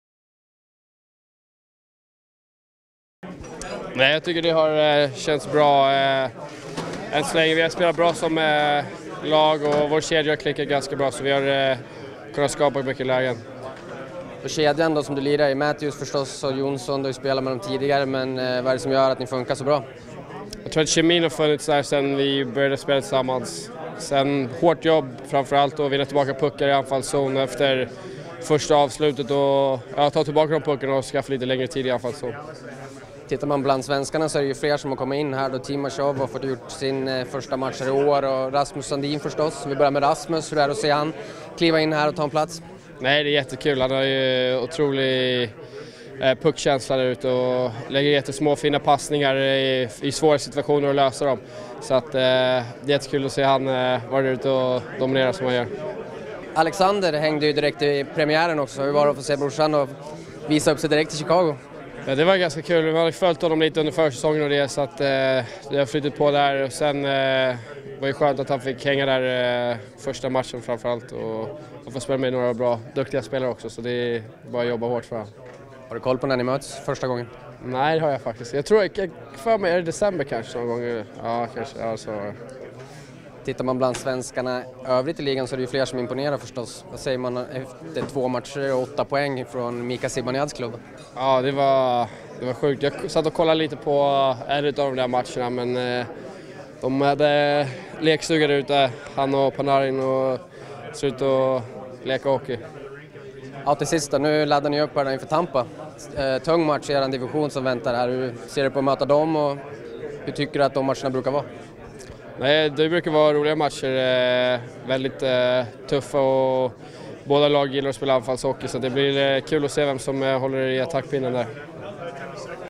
Nylander: "Han och Panarin hade lekstuga"
HockeyNews.se har träffat William Nylander inför nattens match mot Tampa Bay Lightning på hemmaplan.
23-åringen pratar om:
• Brorsans drömstart i Chicago.
• Mika Zibanejad och Artemi Panarins samspel i Rangers.
• Kemin med Matthews och Johnsson.
• Sandins imponerande säsongsöppning.